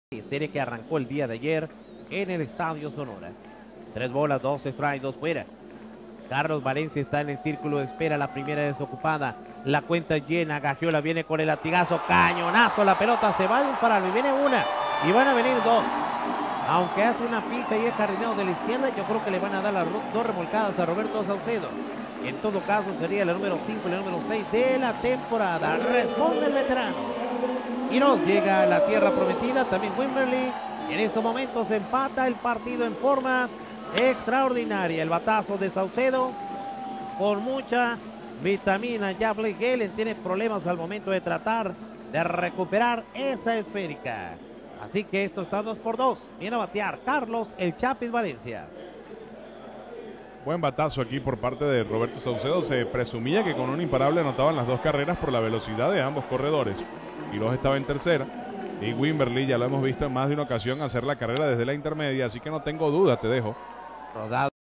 Con sencillo al jardín izquierdo Roberto Saucedo produce dos carreras para empatar el partido.
VIDEO Con sencillo al jardín izquierdo Roberto Saucedo produce dos carreras para empatar el partido.